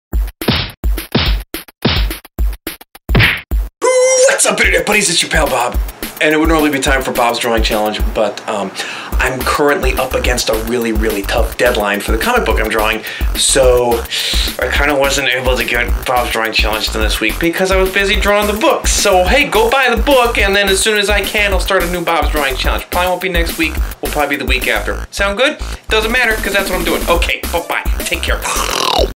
SPECIAL MESSAGE FROM BOB
Too busy making those comics!!! :(((
Bob's drawing challenge will return when I'm done with issue 4.